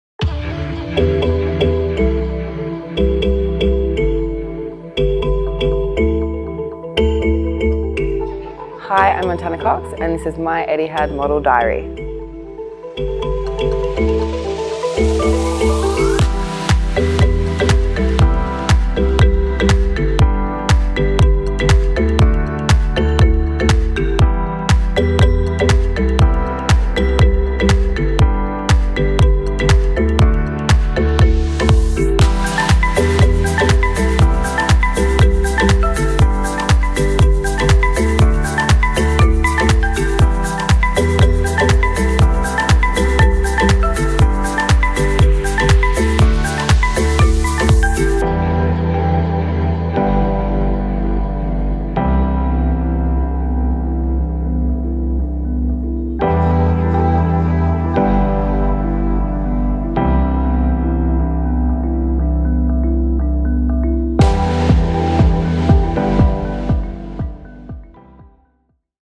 Model Diaries | Montana Cox’s Journey to New York Fashion Week
Journey with fashion model Montana Cox from Sydney to New York Fashion Week.